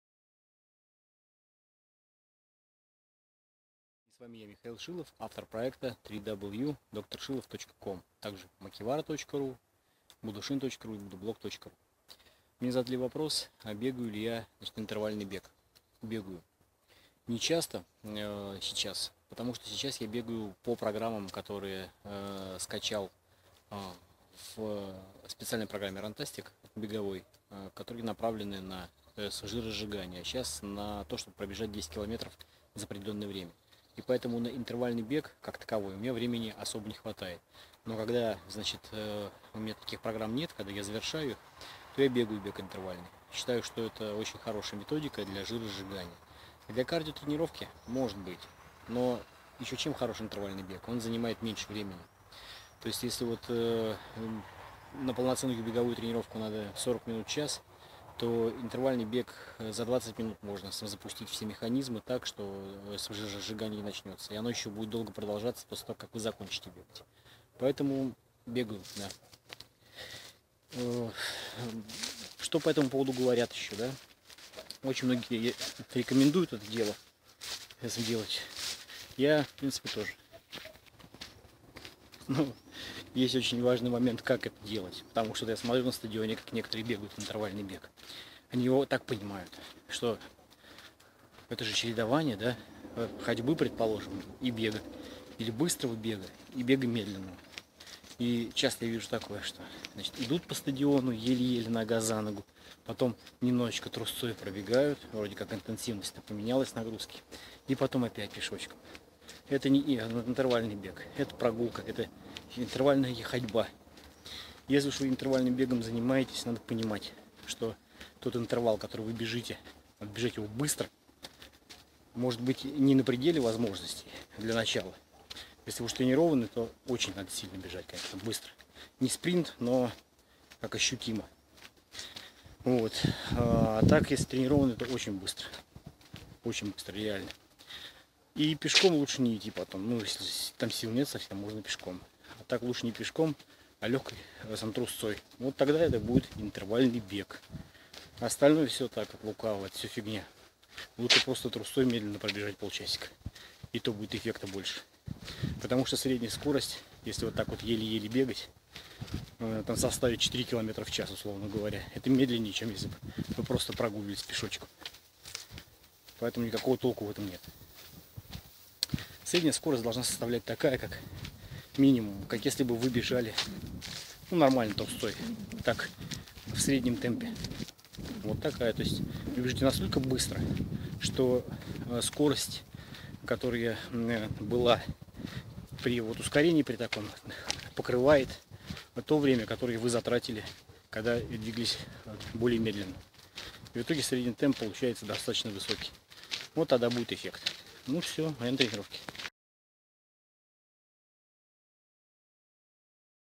Интервальный бег. Проект Корректура. Михаил Шилов
Не имеет смысла превращать интервальный бег в интервальную ходьбу.
Средняя скорость перемещения на тренировке должна быть никак не меньше, чем при целевом беге малым или даже средним темпом. Если средняя скорость перемещения в тренировке интервальным бегом ниже чем 8 км/час, то это уже не бег вовсе.
Ускорение нужно бежать если и не стиле спринта, то уж по-любому выше среднего темпа, а в восстановительном промежутке лучше бежать трусцой или хотя бы быстро идти. Но, уж никак не плестись "нога-за-ногу".